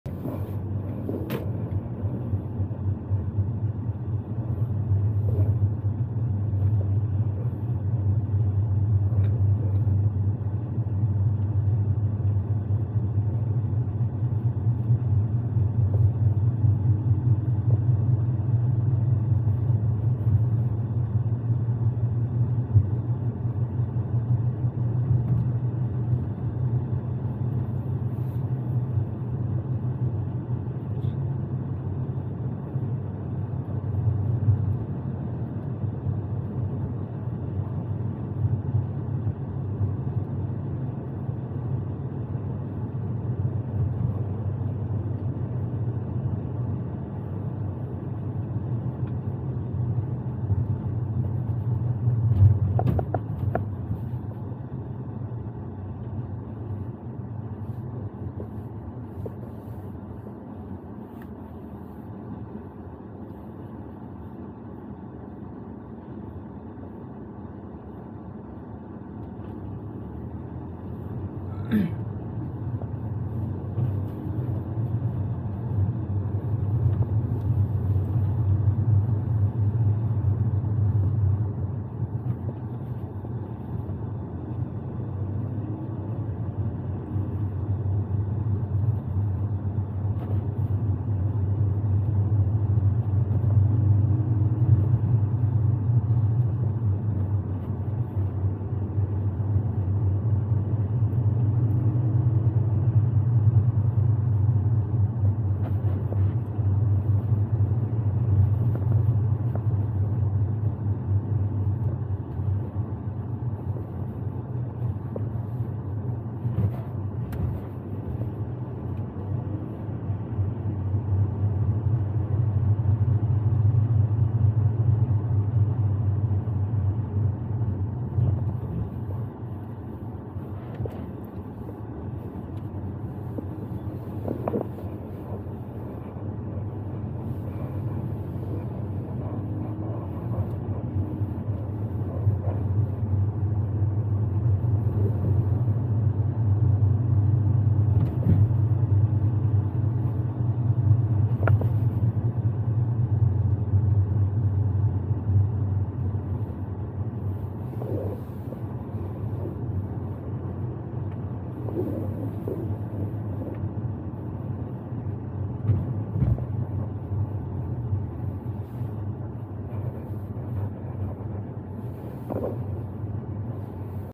가로수 길, Garosugil ,2022-Jan-01(Sat), 07:50am, -9.5℃
가로수길-한강공원, 미세먼지조금, 맑음.
2022년 새해 첫날 가로수길. 아침.